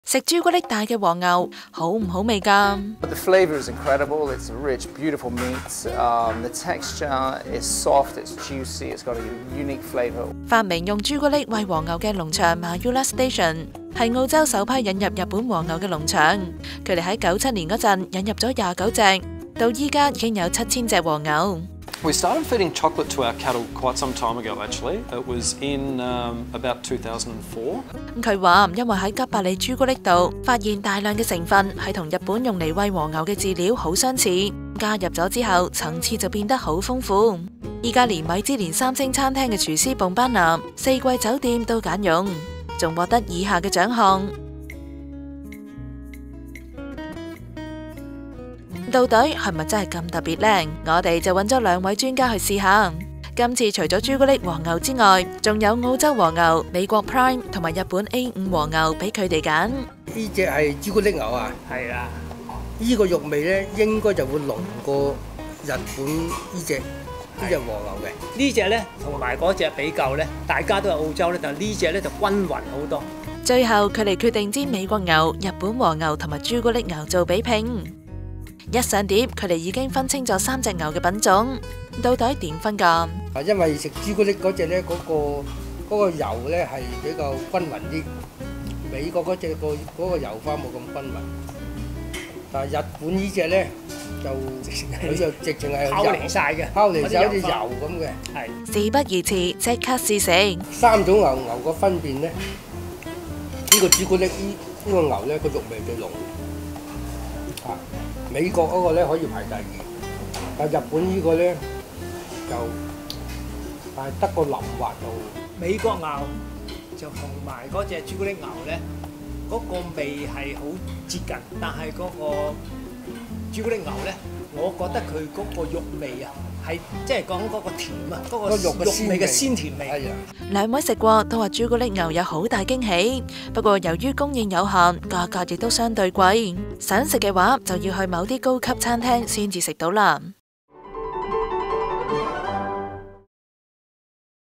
用朱古力養嘅牛牛 澳洲和牛好味過美日牛
牛扒很多種，要突圍而出，不想想法子都不行。這隻來自南澳洲石灰岩海岸的Mayura和牛，負責人Scott De Bruin表示他們是首間引入日本和牛的澳洲農場，當年只有二十九隻和牛，而現在已經擴張至擁有七千隻和牛的大型農場。